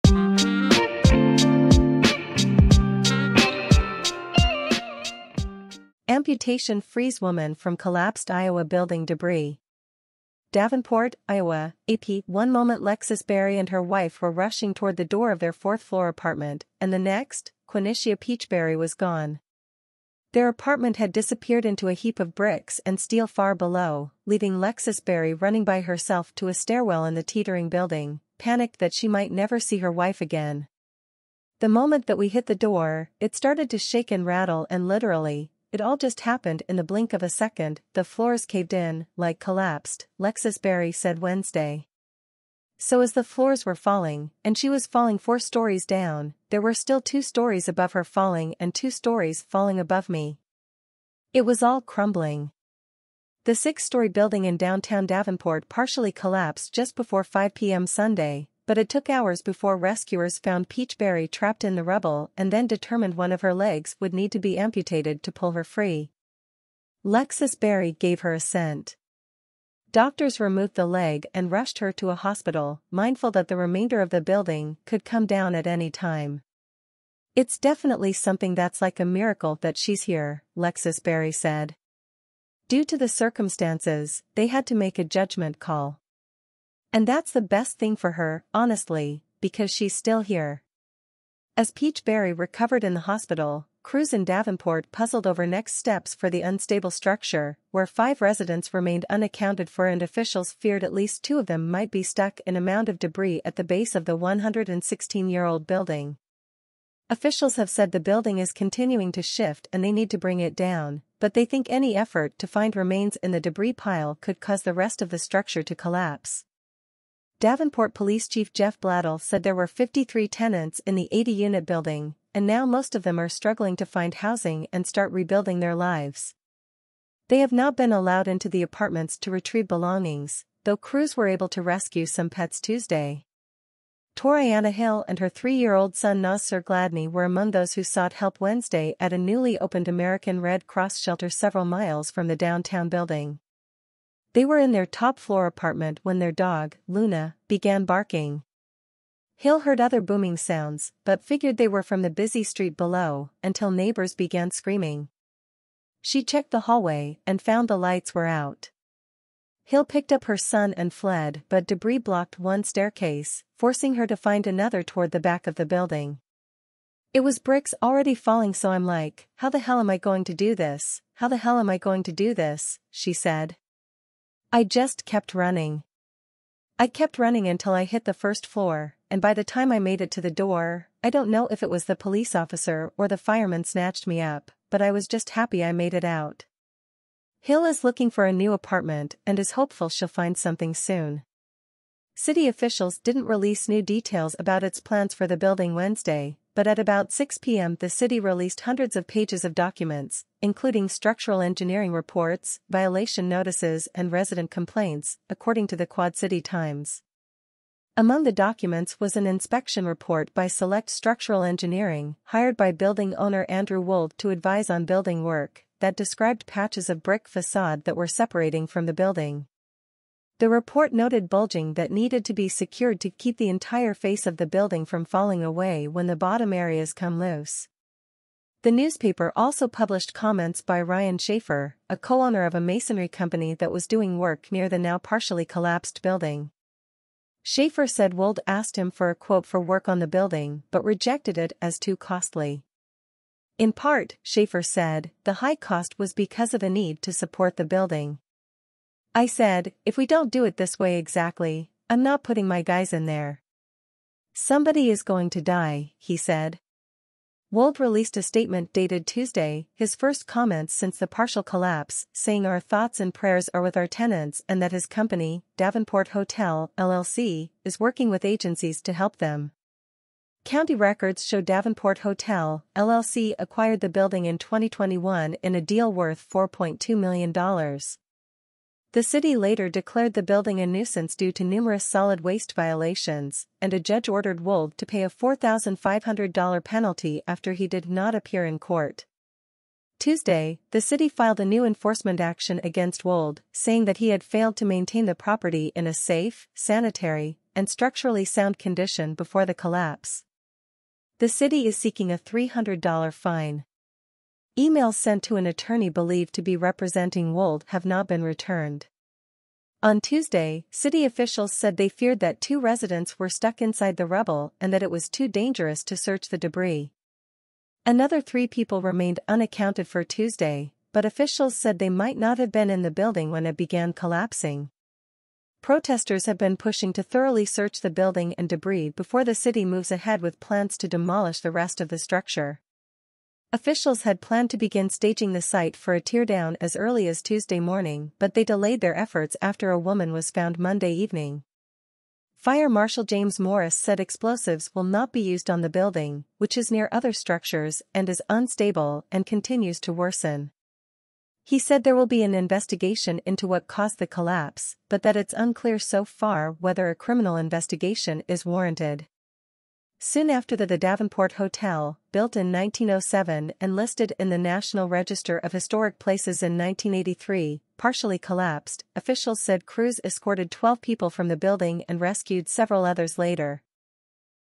Amputation frees woman from collapsed Iowa building debris#news
Amputation frees woman from collapsed Iowa building debris

DAVENPORT, Iowa (AP) — One moment Lexus Berry and her wife were rushing toward the door of their fourth-floor apartment, and the next, Quanishia “Peach” Berry was gone.

Their apartment had disappeared into a heap of bricks and steel far below, leaving Lexis Berry running by herself to a stairwell in the teetering building, panicked that she might never see her wife again.

“The moment that we hit the door, it started to shake and rattle and literally — it all just happened in the blink of a second – the floors caved in, like collapsed,” Lexus Berry said Wednesday. “So as the floors were falling, and she was falling four stories down, there were still two stories above her falling and two stories falling above me. It was all crumbling.”

The six-story building in downtown Davenport partially collapsed just before 5 p.m. Sunday, but it took hours before rescuers found Peach Berry trapped in the rubble and then determined one of her legs would need to be amputated to pull her free. Lexus Berry gave her assent. Doctors removed the leg and rushed her to a hospital, mindful that the remainder of the building could come down at any time.

“It’s definitely something that’s like a miracle that she’s here,” Lexus Berry said. “Due to the circumstances, they had to make a judgment call. And that’s the best thing for her, honestly, because she’s still here.”

As Peach Berry recovered in the hospital, crews in Davenport puzzled over next steps for the unstable structure, where five residents remained unaccounted for and officials feared at least two of them might be stuck in a mound of debris at the base of the 116-year-old building. Officials have said the building is continuing to shift and they need to bring it down, but they think any effort to find remains in the debris pile could cause the rest of the structure to collapse.

Davenport Police Chief Jeff Bladel said there were 53 tenants in the 80-unit building, and now most of them are struggling to find housing and start rebuilding their lives. They have not been allowed into the apartments to retrieve belongings, though crews were able to rescue some pets Tuesday.

Toriana Hill and her 3-year-old son Nassir Gladney were among those who sought help Wednesday at a newly opened American Red Cross shelter several miles from the downtown building.

They were in their top-floor apartment when their dog, Luna, began barking. Hill heard other booming sounds but figured they were from the busy street below, until neighbors began screaming. She checked the hallway and found the lights were out.

Hill picked up her son and fled but debris blocked one staircase, forcing her to find another toward the back of the building.

“It was bricks already falling so I’m like, ‘how the hell am I going to do this, how the hell am I going to do this?’” she said. “I just kept running. I kept running until I hit the first floor, and by the time I made it to the door, I don’t know if it was the police officer or the fireman snatched me up, but I was just happy I made it out.”

Hill is looking for a new apartment and is hopeful she’ll find something soon.

City officials didn’t release new details about its plans for the building Wednesday, but at about 6 p.m. the city released hundreds of pages of documents, including structural engineering reports, violation notices and resident complaints, according to the Quad-City Times.

Among the documents was an inspection report by Select Structural Engineering, hired by building owner Andrew Wold to advise on building work, that described patches of brick façade that were separating from the building. The report noted bulging that needed to be secured to “keep the entire face of the building from falling away when the bottom area(s) come loose.”

The newspaper also published comments by Ryan Shaffer, a co-owner of a masonry company that was doing work near the now-partially collapsed building. Shaffer said Wold asked him for a quote for work on the building but rejected it as too costly. In part, Shaffer said, the high cost was because of a need to support the building.

“I said, ‘If we don’t do it this way exactly, I’m not putting my guys in there. Somebody is going to die,’ ” he said.

Wold released a statement dated Tuesday, his first comments since the partial collapse, saying “our thoughts and prayers are with our tenants” and that his company, Davenport Hotel, L.L.C., is working with agencies to help them.

County records show Davenport Hotel, L.L.C. acquired the building in 2021 in a deal worth $4.2 million. The city later declared the building a nuisance due to numerous solid waste violations, and a judge ordered Wold to pay a $4,500 penalty after he did not appear in court.

Tuesday, the city filed a new enforcement action against Wold, saying that he had failed to maintain the property “in a safe, sanitary, and structurally sound condition” before the collapse.